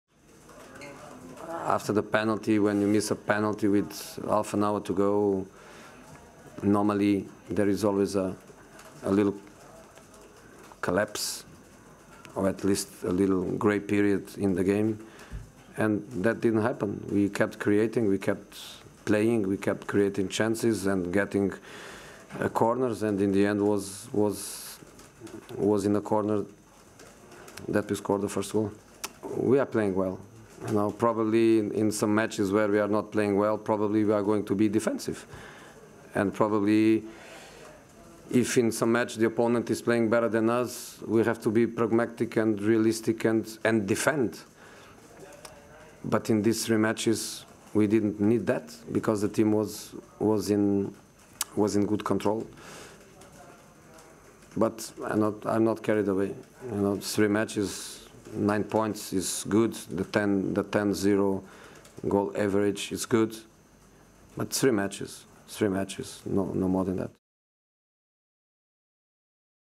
Mourinho not getting carried away after Manchester United maintain 100% record
José Mourinho says he is not getting carried away with Manchester United's good start to the new season, despite his side's 100% record in the Premier League. United saw off Leicester 2-0 at Old Trafford on Saturday thanks to goals from Marcus Rashford and Marouane Fellaini